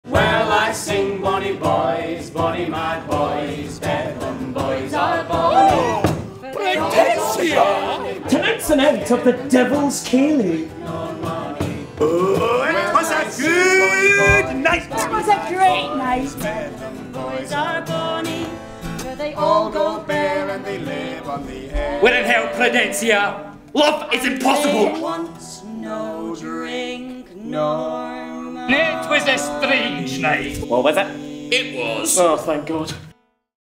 Prudencia Hart | 2018 trailer
Pull up a chair and whet your whistle for an evening of anarchic theatre, live music and strange goings-on…

One wintry morning Prudencia Hart, an uptight academic, sets off to attend a conference in Kelso in the Scottish Borders. As the snow begins to fall, little does she know who or what awaits her there. Inspired by the border ballads – and delivered in a riotous romp of rhyming couplets, devilish encounters and wild karaoke – Prudencia’s dream-like journey of self-discovery unfolds among and around the audience.

With a rollicking text by David Greig, barnstorming live music; and a ‘terrific, inventive sense of fun…’ (The Scotsman), The Strange Undoing of Prudencia Hart is the lock-in to end all lock-ins.

National Theatre of Scotland cannot be held responsible in the event of any member of the audience losing their head, their heart or their very self during the course of the performance.

Recommended age guide – 14+

Created by David Greig (writer) and Wils Wilson (director)